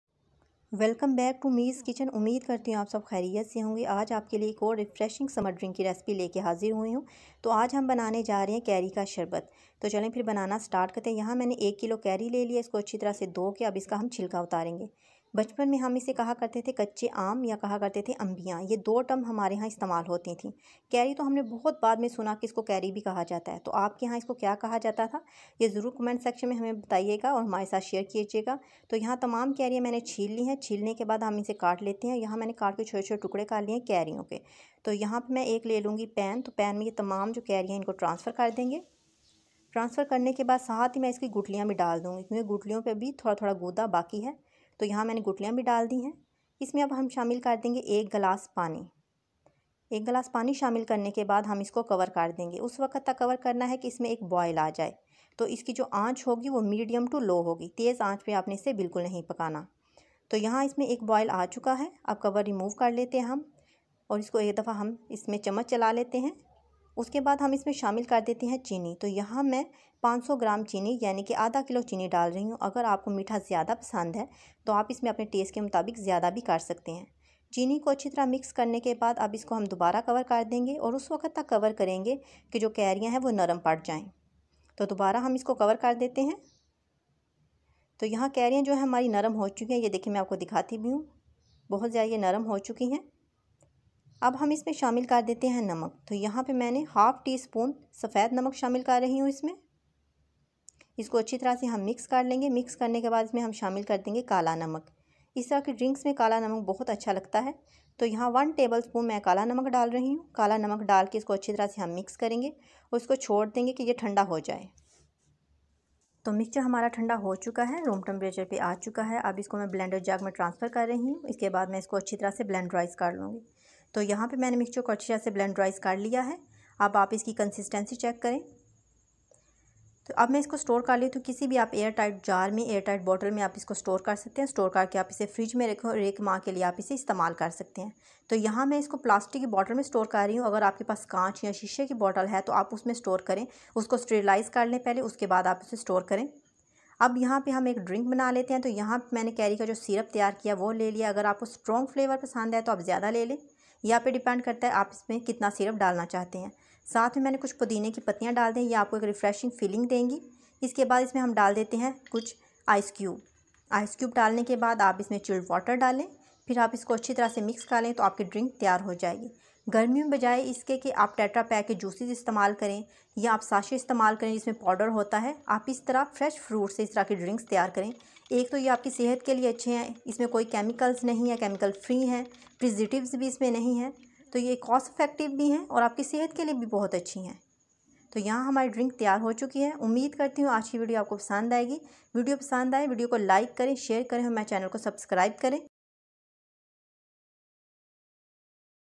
Keri Ka Sharbat Recipe|Aam Panna Recipe|Kacche Aam ka sharbat Recipe|Raw Mango Drink|Meers kitchen
Today,I'm sharing with you a very refreshing summer drink recipe.